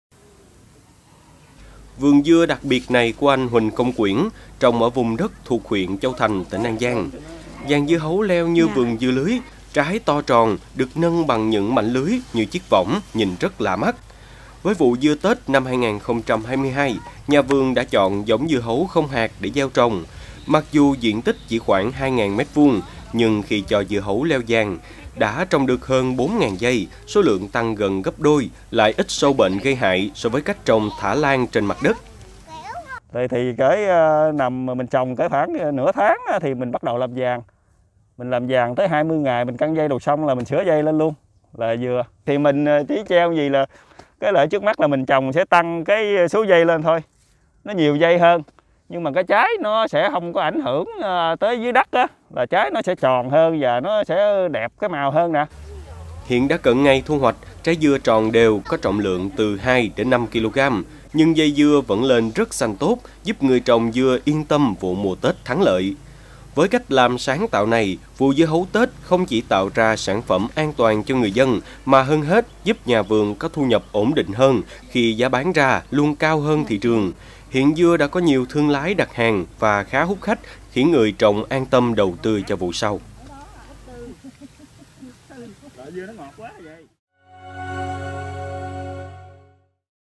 Dưa hấu Tết to, tròn hơn nhờ được 'nằm võng'
Xưa nay người trồng dưa hấu thường thả cho dây dưa bò trên mặt đất, nhưng ở An Giang có một nông dân nghĩ ra cách ‘mắc võng’ cho dưa như cách trồng dưa lưới. Cách làm này giúp trái dưa hấu to, tròn hơn nhờ đó năng suất vượt trội.